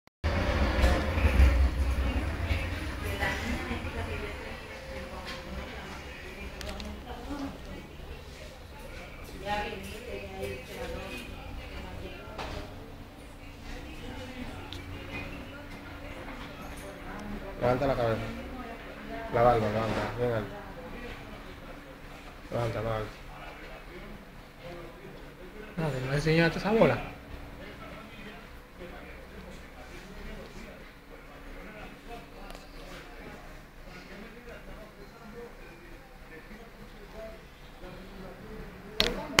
CORTE DE PELO EL DESCUIDO.PIEL IRRITADA
UNA PIEL DELICADA ES UNA  DE LAS COSAS QUE TE IDENTIFICA  COMO PELUQUERO.movil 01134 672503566